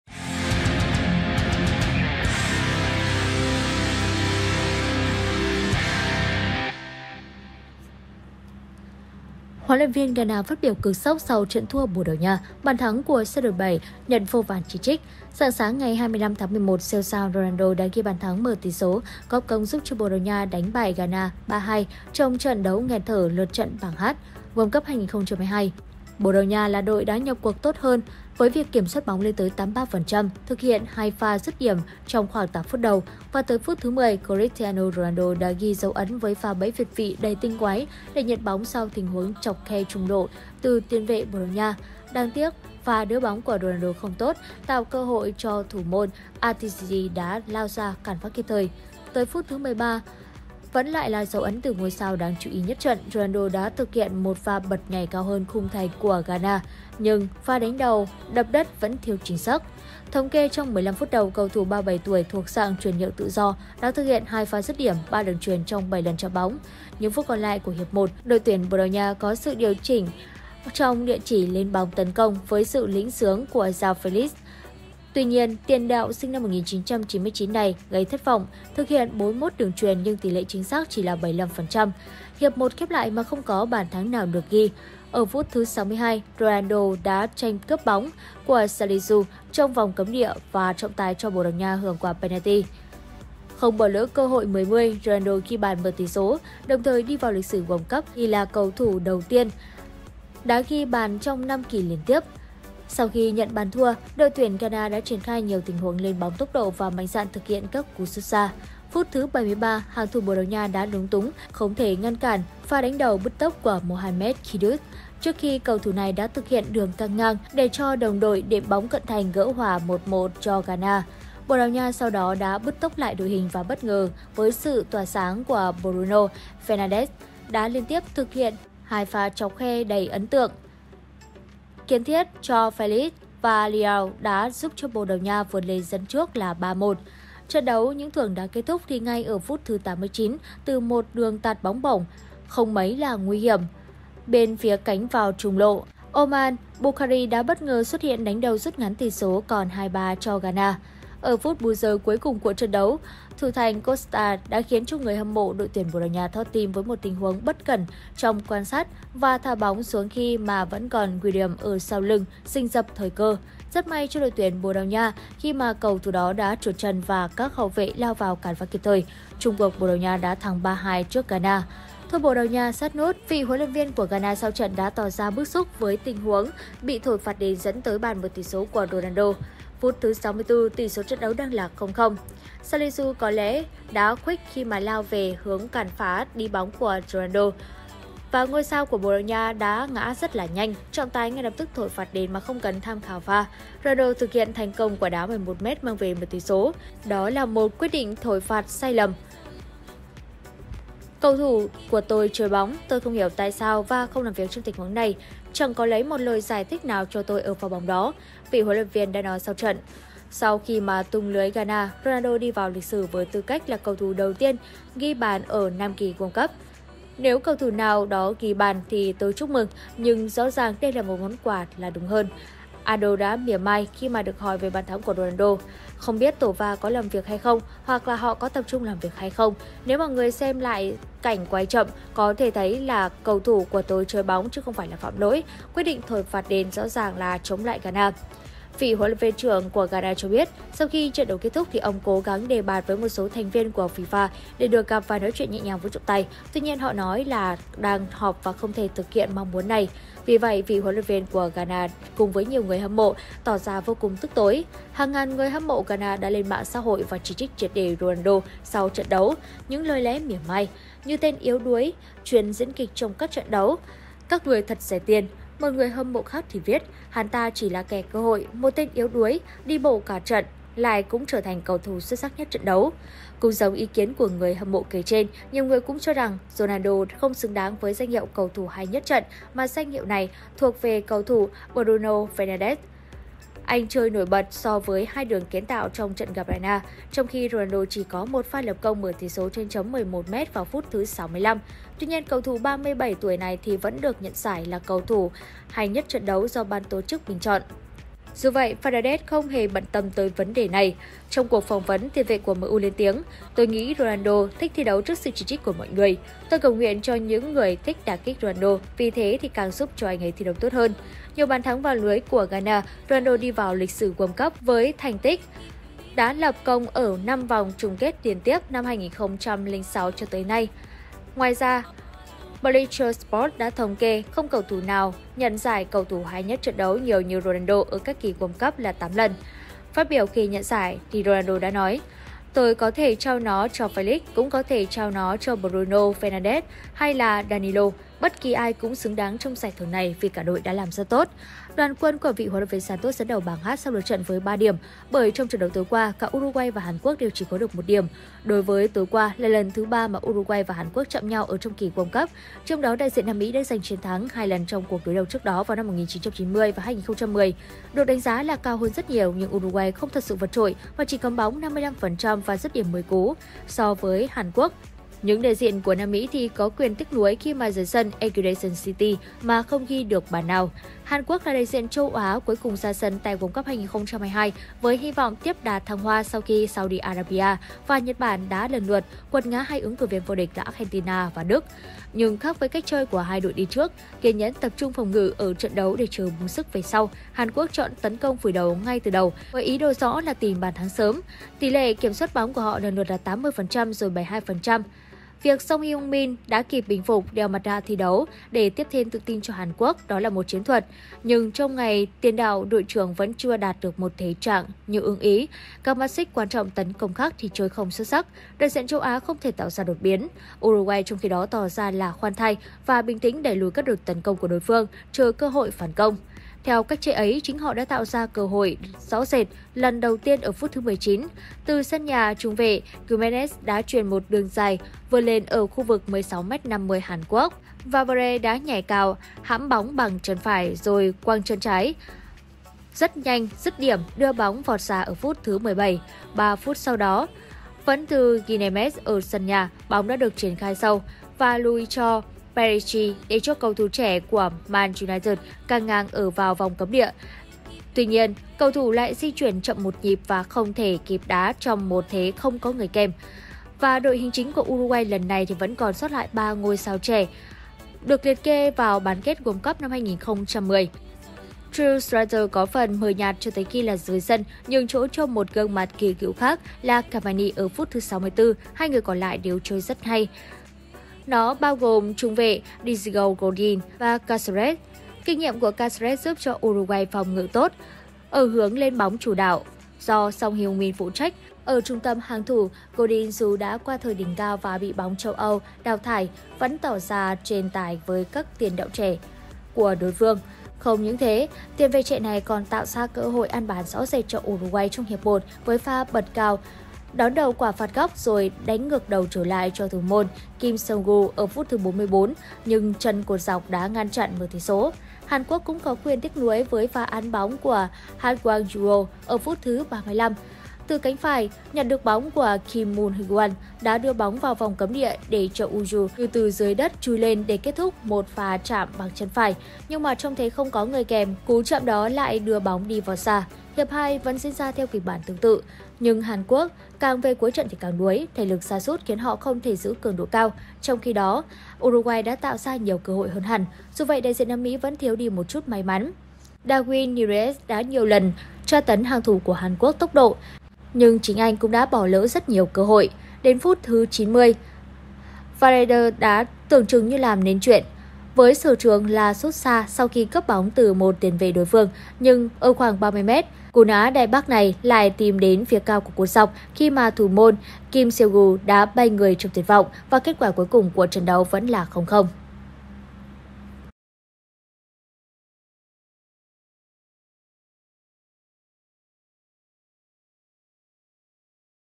HLV Ghana "Tức Giận" phát biểu Chấn Động ở trận thua Bồ Đào Nha, bàn thắng của Ronaldo nhận gạch đá
HLV Ghana "Tức Giận" phát biểu Chấn Động sau trận thua Bồ Đào Nha, bàn thắng của Ronaldo nhận gạch đá